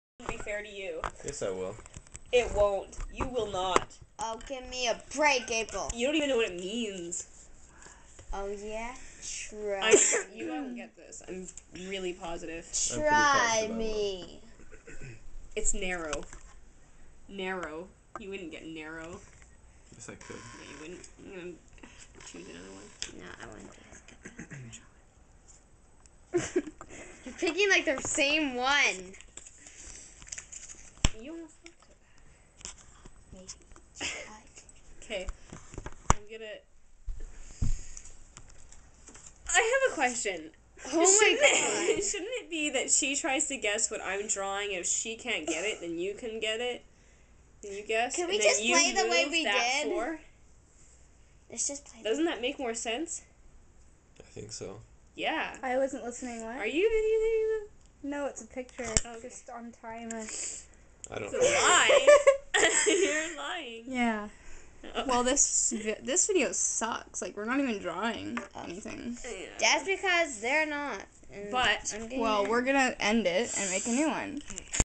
xmas vids 1- April making the rules for pictionary.
April making the rules for pictionary.